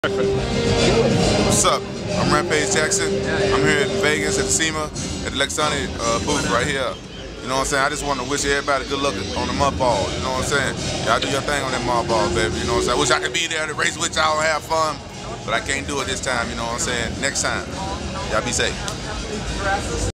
Rampage Jackson Wishing Modballers Good Luck
A good luck message from former UFC Light Heavyweight to you Modballers... he'll be joining you all in 2012!!